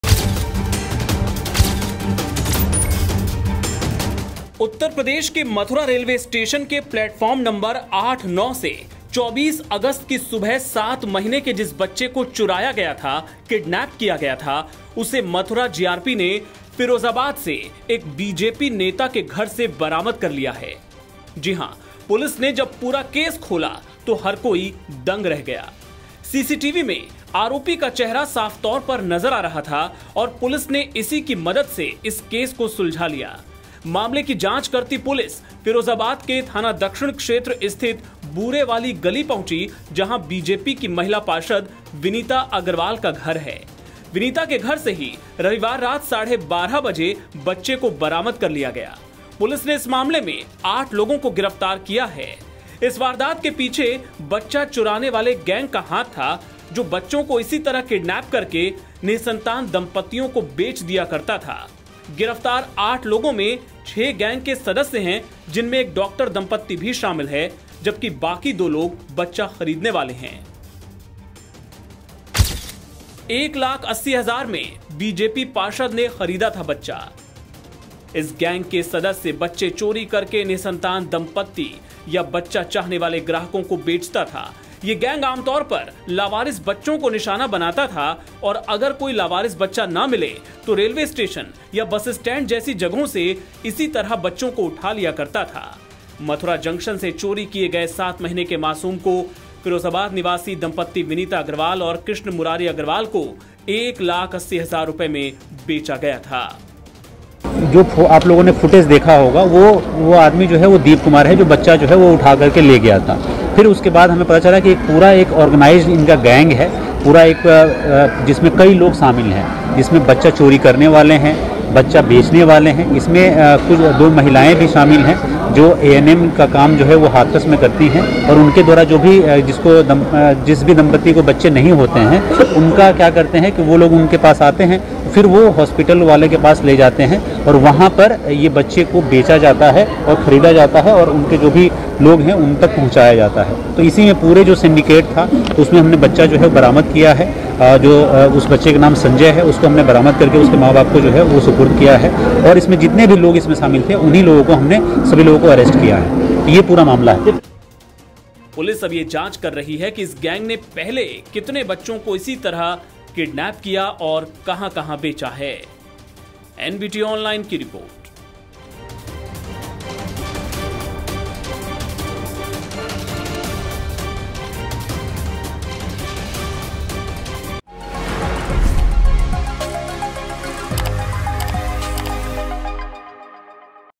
Mathura Railway Station से Kidnap बच्चा Firozabad BJP पार्षद Vineeta Agrawal के घर से बरामद। CCTV
उत्तर प्रदेश के मथुरा रेलवे स्टेशन के प्लेटफार्म नंबर 8/9 से 24 अगस्त की सुबह सात महीने के जिस बच्चे का चुराया गया था, उसे मथुरा जीआरपी ने फिरोजाबाद से एक बीजेपी नेता के घर से बरामद कर लिया है। जी हां, पुलिस ने जब पूरा केस खोला, तो हर कोई हैरान रह गया। सीसीटीवी में आरोपी का चेहरा साफ नजर आ रहा था और पुलिस ने इसी की मदद से इस केस को सुलझाया। मामले की जांच करती पुलिस फिरोजाबाद के थाना दक्षिण क्षेत्र स्थित बूरे वाली गली पहुंची, जहां बीजेपी की महिला पार्षद विनीता अग्रवाल का घर है। विनीती के घर से ही रविवार रात साढ़े 12 बजे बच्चे को बरामद कर लिया। पुलिस ने इस मामले में 8 लोगों को गिरफ्तार किया है। इस वारदात के पीछे बच्चा चुराना वाला गैंग था जो बच्चा चुरा कर निसंतान दंपत्तियों को बेचने का काम करते था। गिरफ्तार 8 लोगों में 6 गैंग के सदस्य हैं, जिनमें एक डॉक्टर दंपती भी शामिल है और बाकी 2 लोग बच्चा खरीदने वाले हैं।